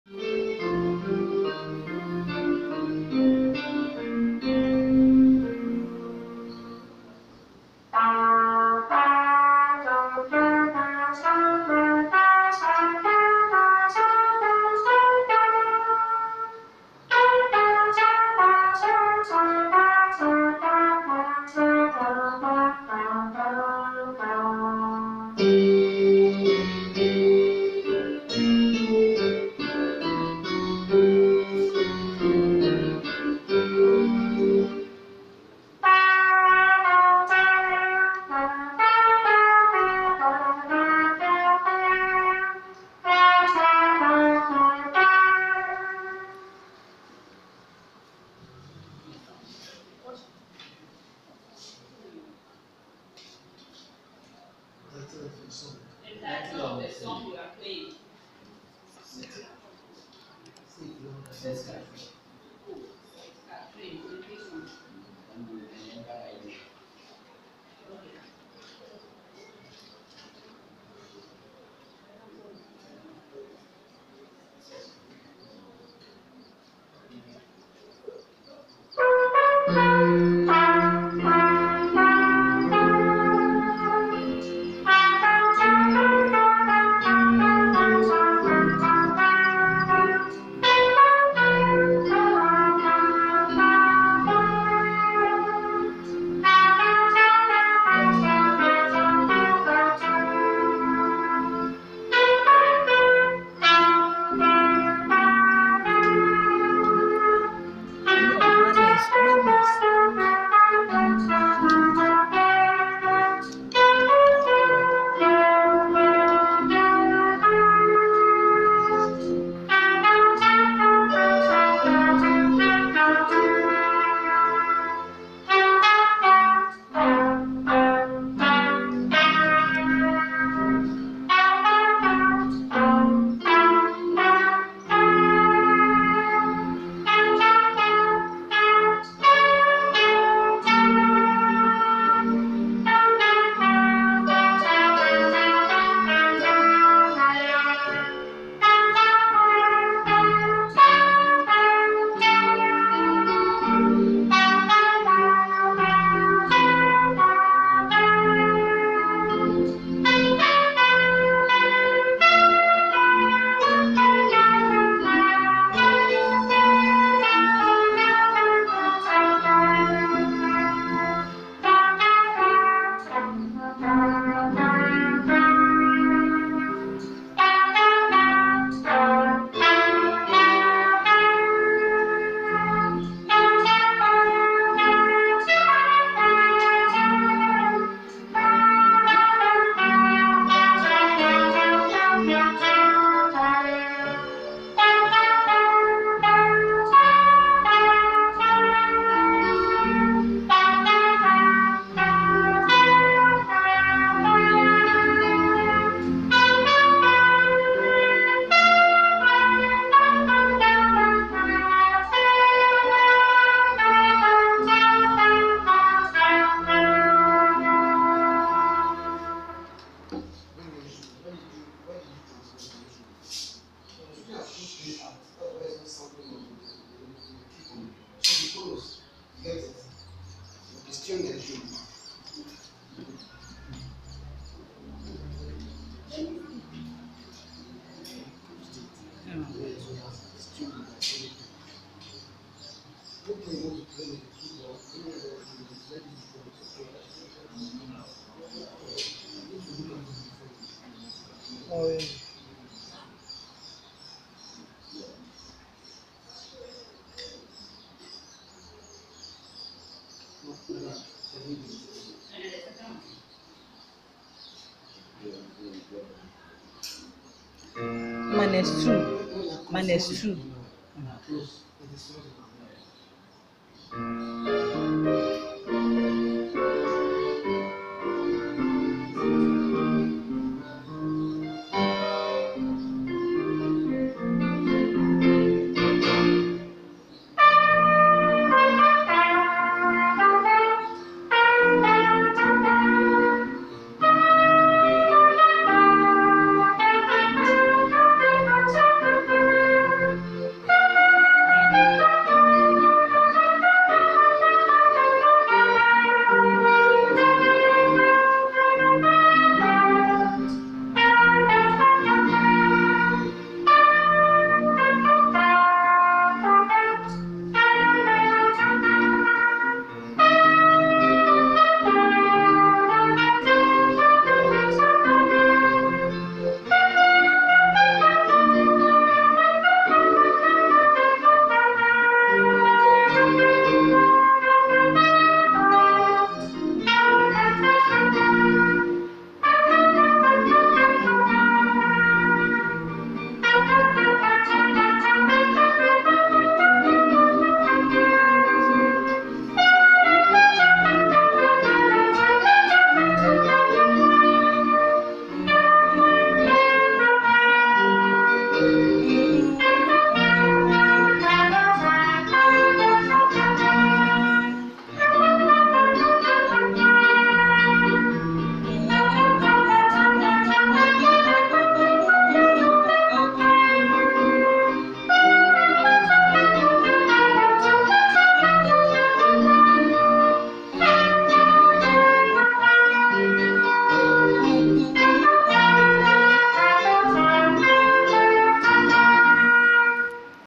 Kojo Trumpet WASSCE performance2019@Dzosec
Hormenoo Christ(Kojo_trumpet) performance on his final examination in Dzodze Penyi senior high school.
On that faithful day his instrument(trumpet)was distuned by fellow school mates before the practical exams. The pianist had a problem tunning to his key.
But they later realized that after his first song played through.  😔